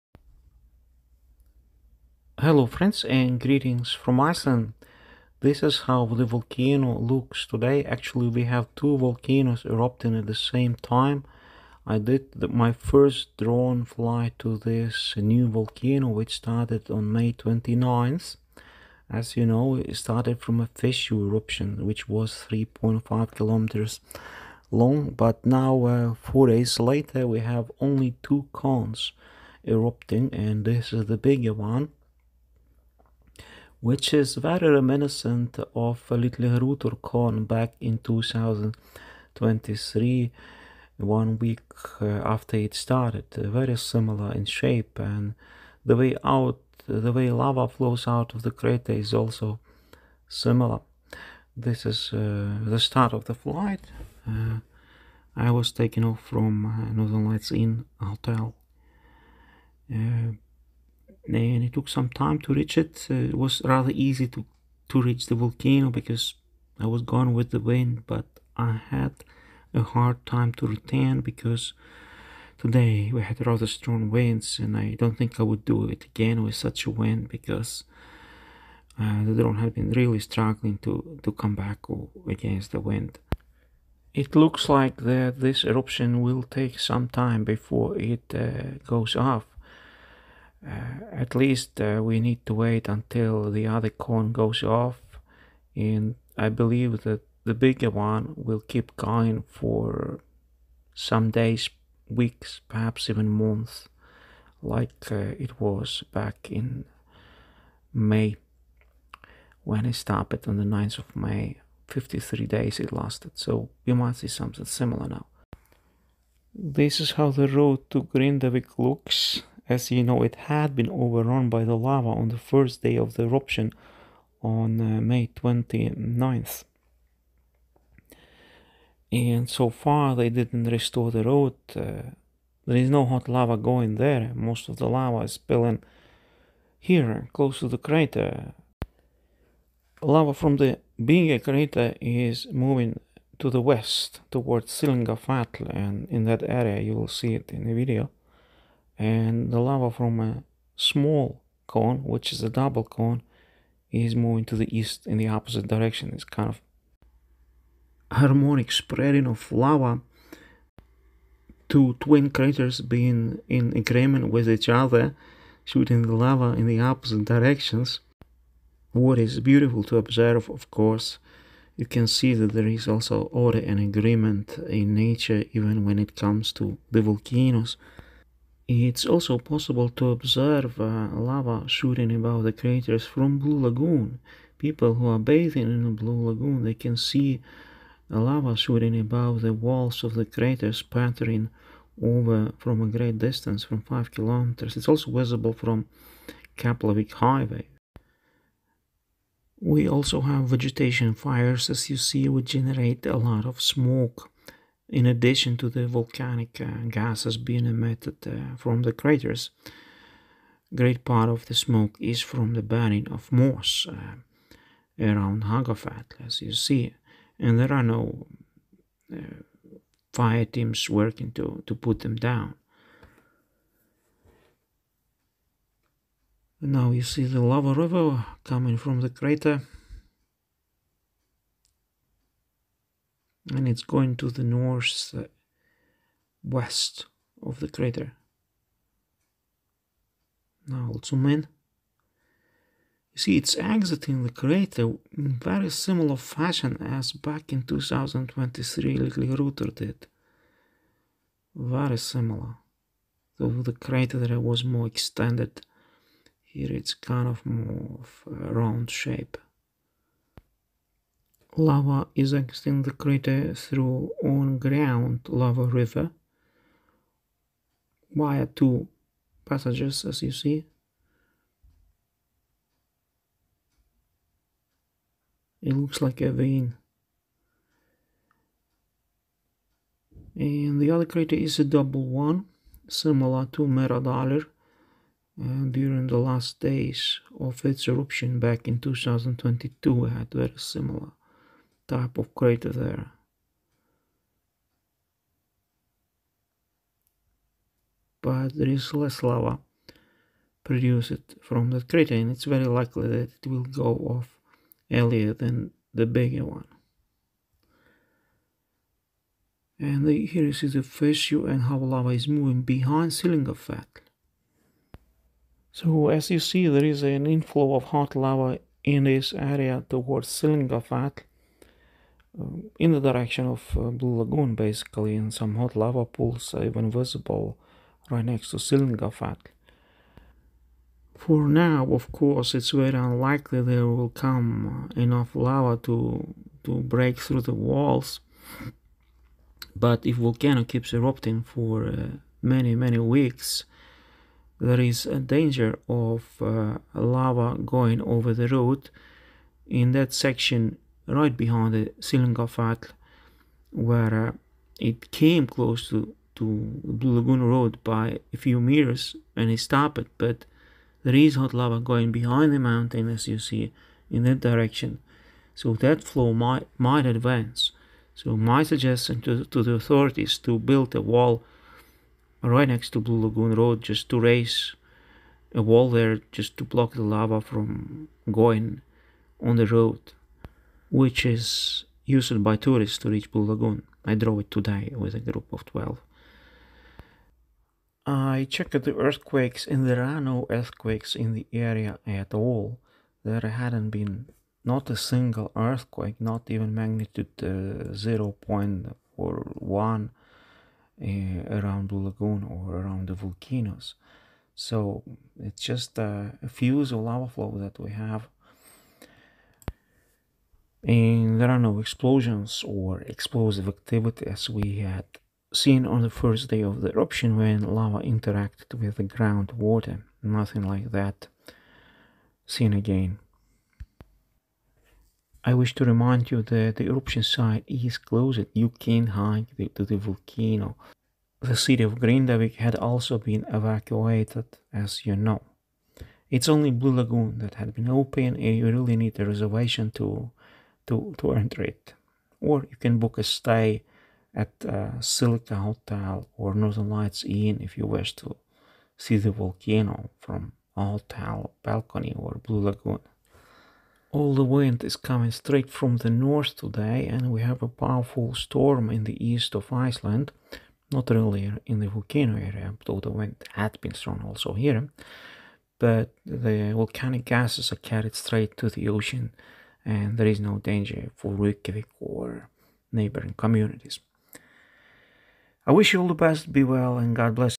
Two Craters Erupting now. Northern cone is a twin crater. Drone explores new volcanoes. 4K 03.06
Volcano update from Iceland. Lava encircles Sylingafell mountain and might push towards the road to Blue Lagoon (road 43). The southern crater is likely to last for some weeks.  The Southern cone also emits lava to the south (from its backside) via a tube, where we see a lava pool.

5th Volcanic Eruption (8th since 2021) of Reykjanes Volcano started on May 29th, 2024 at 12:50 just north of Grindavik, Reykjanes Peninsula. As for 16:00 May 29th the erupting fissure was 3.4 km long and the lava ran over the road no. 43 (Grindavik road) for the 2nd time in that location (first time back in January) and also over road 425 (Nesvegur) west of town. By the evening of the first day activity subsided and was concentrated along some 2 km stretch, more or less at the same location as back in March, around the old crater. As for today, June 3 we have two distinct cones erupting close distance to each other, emitting lava flows in the opposite directions: the northern cone emits lava to the east, while the southern one (bigger one) sends lava to the west where its facing a mountain (Sylingafell). Southern cone also emits lava to the south via a tube, where we see a lava pool.

Update (05.06.24): As predicted, the northern cone (with twin vents) nearly died out over last night.

+++ THANK YOU! +++ TAKK FYRIR  +++ MEGI GUÐ ALMÁTTUGUR BLESSI ÍSLAND OG YKKUR ÖLL! +++